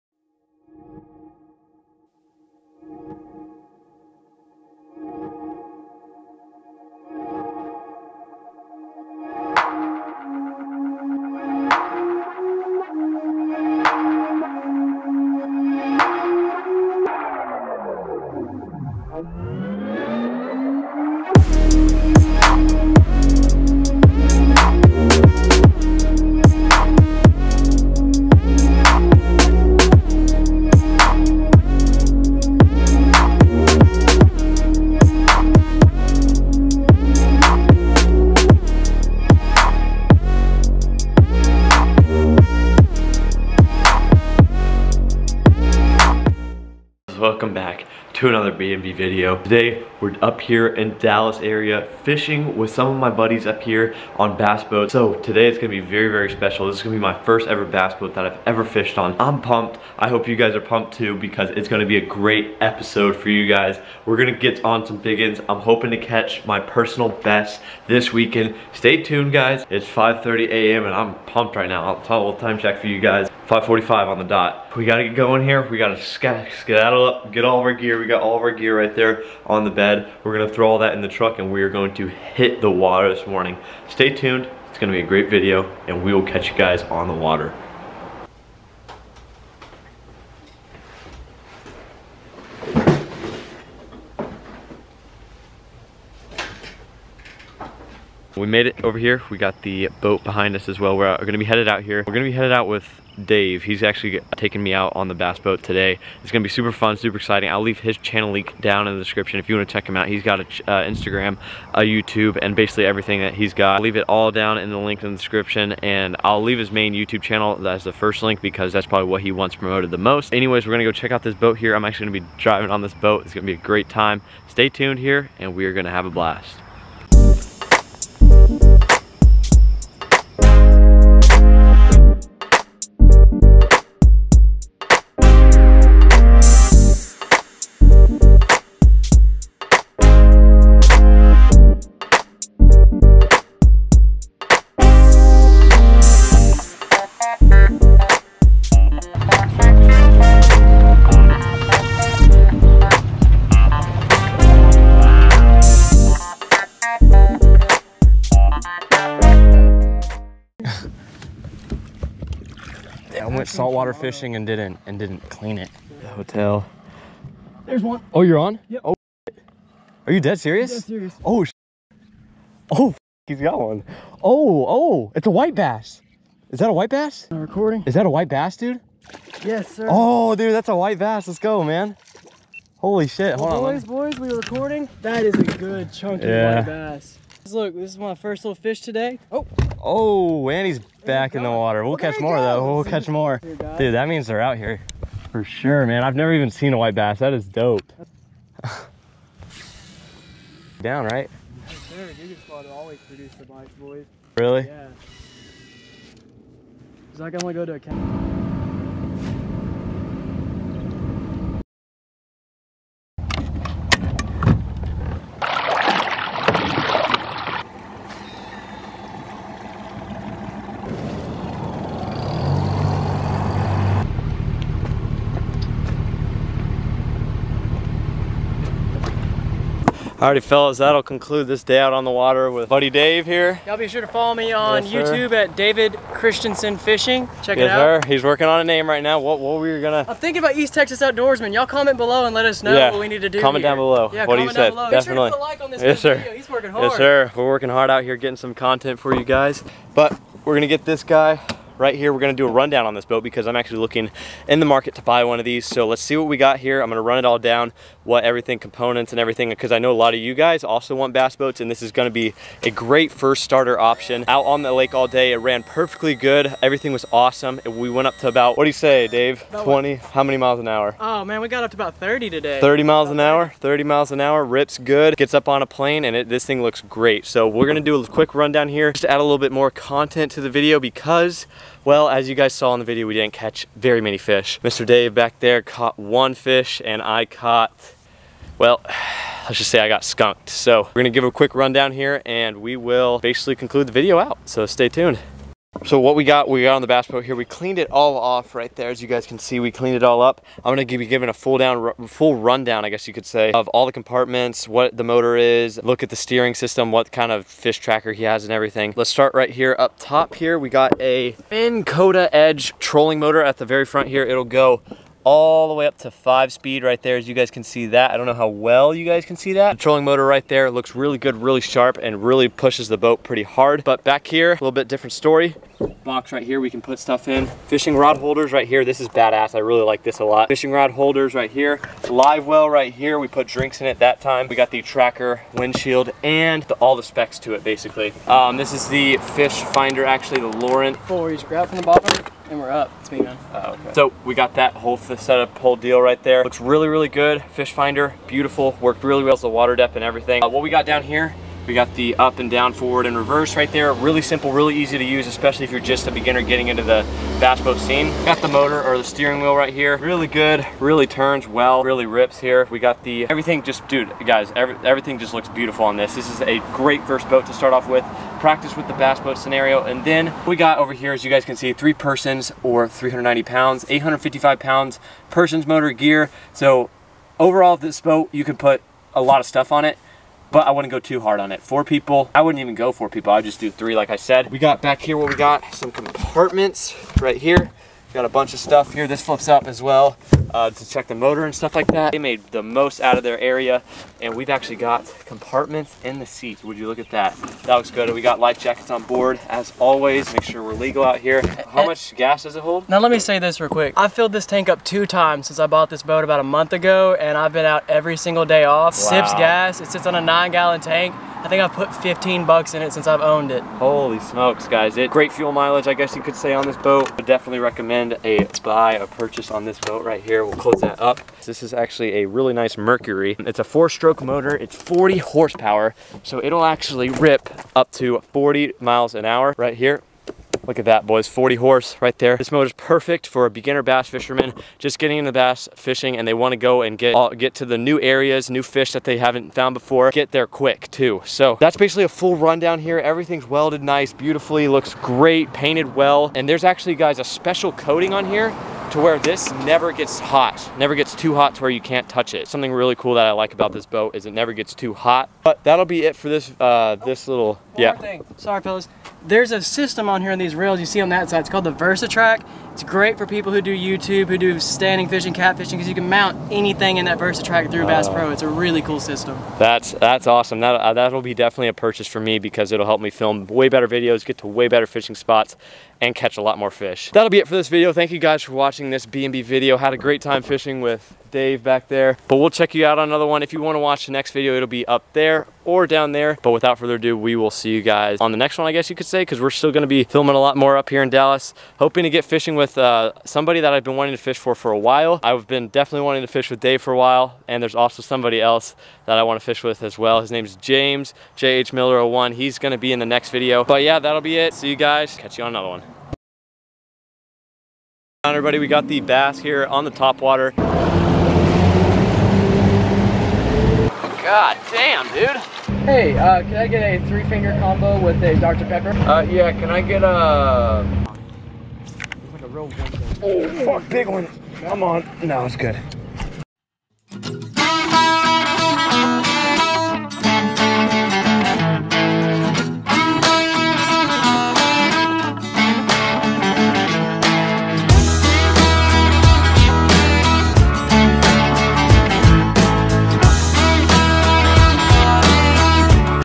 My First Time On A BASS BOAT!! BASS TRACKER Pro 160! (Fishing Lake Fork)
Bass Boat cover to protect from sun

Finally got this video finished and edited. a ton of time and dedication went into this video boys probably the most time money and energy I have ever spent on a video. made a trip all the way up to Dallas by myself to film with some buddies up there had an amazing time even though I barely caught any fish. for those of you who are wondering about the bass boat I was fishing on it is a super affordable bass tracker classic XL they go for around $12,000-$15,000 which is extremely affordable for a bass boa. here are all the specs for the bass boat.
Backed by the TRACKER® PROMISE—the best factory warranty in aluminum boats
DIAMOND COAT Black Diamond or Royal Red finish w/industry-exclusive powder coat & clearcoat for brilliant durability
Reliable & efficient Mercury FourStroke outboard
Factory rigged w/a Minn Kota trolling motor & Lawrence color fishfinder
Rugged, all-welded, 1-piece aluminum hull w/pressed-in strakes & chines
Generous storage below driver & passenger seats, under bow deck & in aft deck
2 folding, TRACKER-made fishing seats w/bow & aft seat bases
9-gal. (34.07 L) aerated, rotomolded livewell in front of console
Port side rod holders w/organizer for 4 rods to 7' (2.13 m)
Custom-matched TRACKER-made trailer w/GALVASHIELD Impact protection

so as you guys can tell by the specs this boat is definitely worth investing the time to take a look. thank you guys for watching and hope you guys enjoyed the video if you want to check out Daves youtube channel here is the link to it